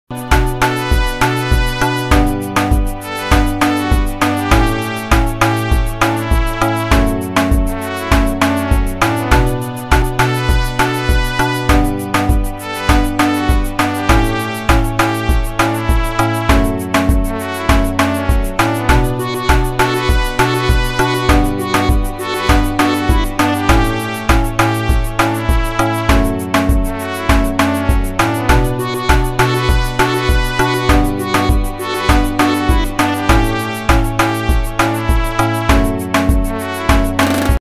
South African girl slowly but surely killing the song^_^😃😀
Best MZANSI dance moves

You can find them only in South Africa 🇿🇦😀